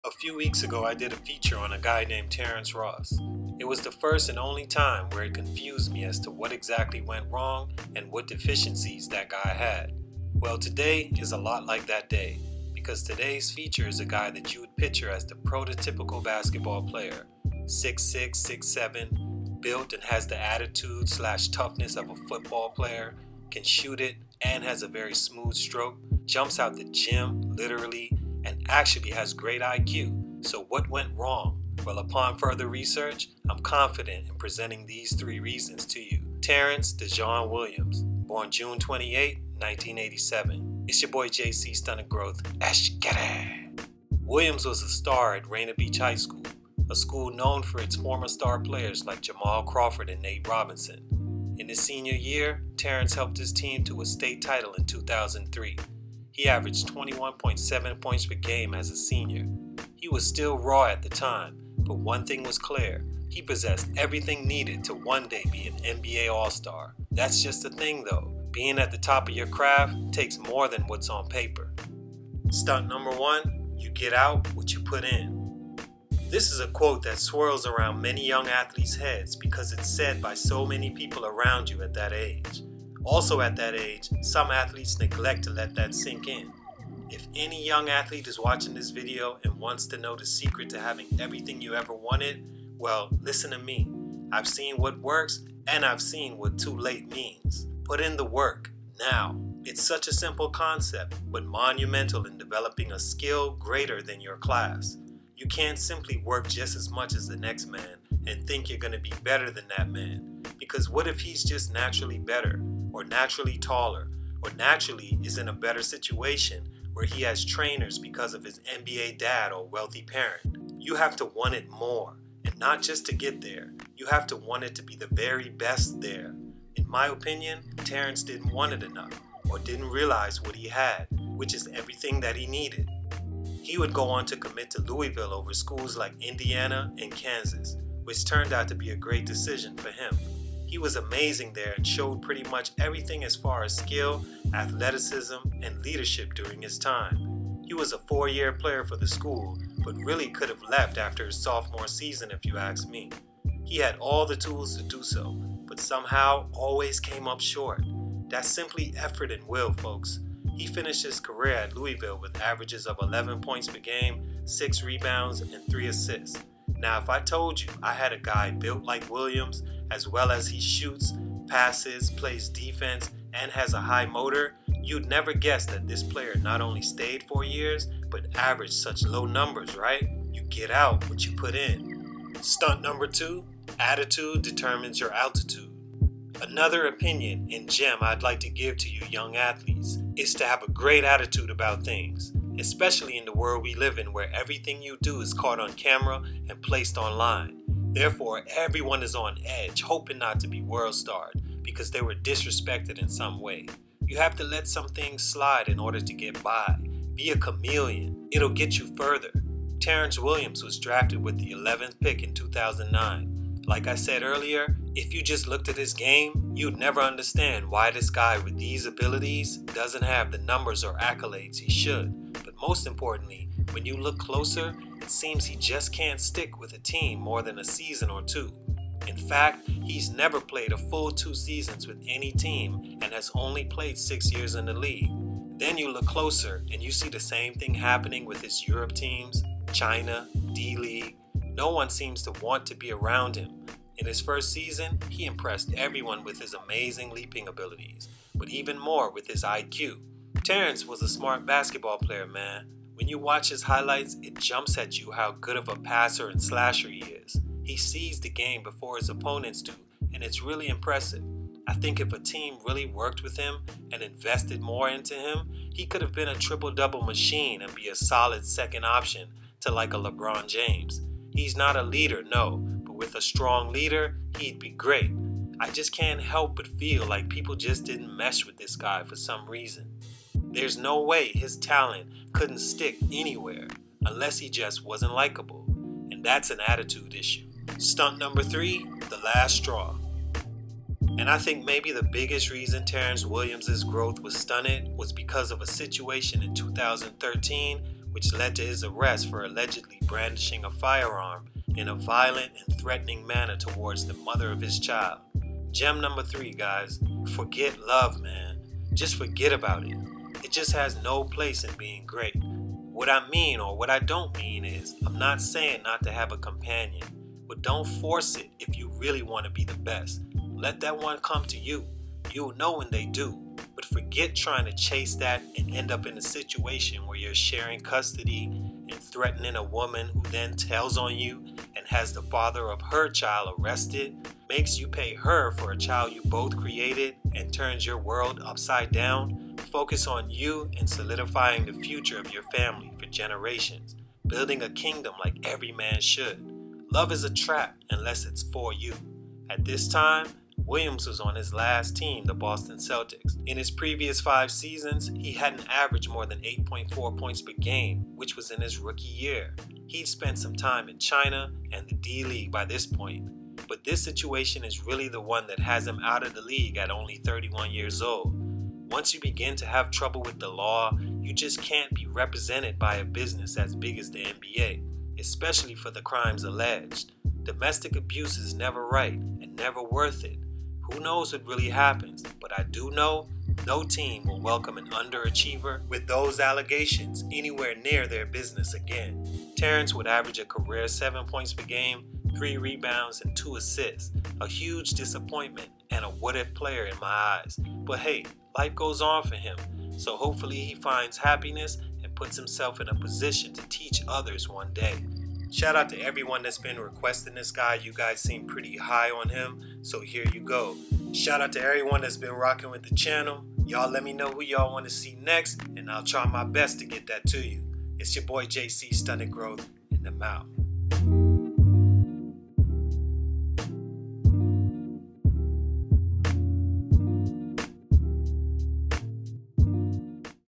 TERRENCE WILLIAMS had game! There's really no reason BUT THESE!
As professional athletes coming into any professional sports, you don't get to determine where you will play for the first years of your career. You don't get to pick your coach, teammates, GM or fans like you do in college. Sometimes it works out for you and sometimes it doesn't. Here's an athlete it didn't workout for as of now. Here's 3 reasons why I think their growth was abruptly stunted. Who knows, maybe they'll return to form. I sure hope so.

Let me know who I should do next! JC, Stunted Growth.

Donating will help me bring you content more often at better quality. Anything will help. Thank you for your time.